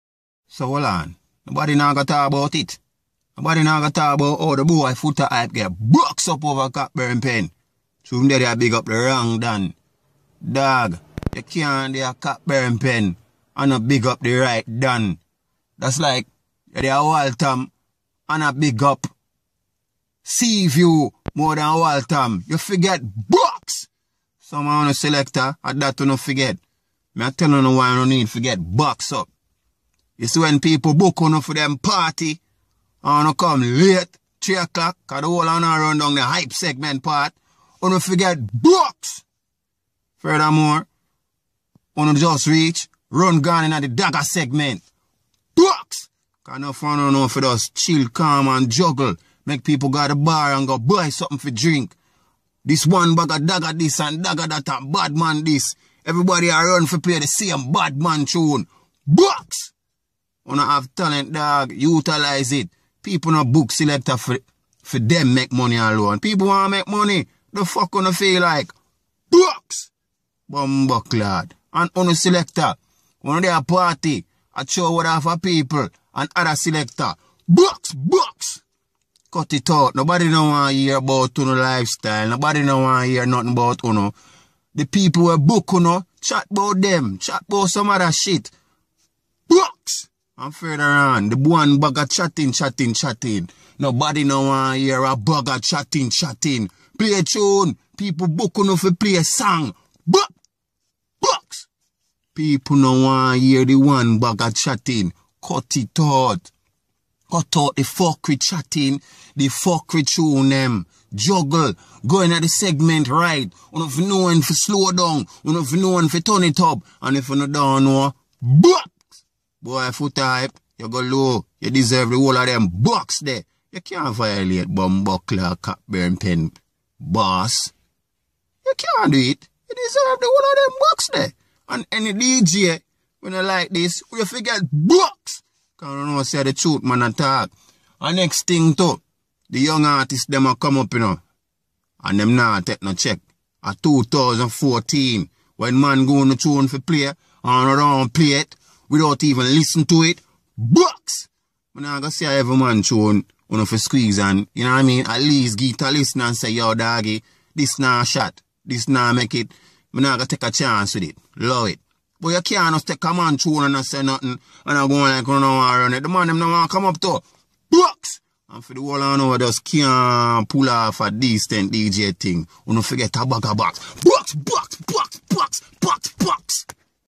WAR PROFESSA - Foota hype get Box Up a Cockburn Pen 2014
war professa talks about how foota hype get beaten in his face in cockburn pen while performing his set.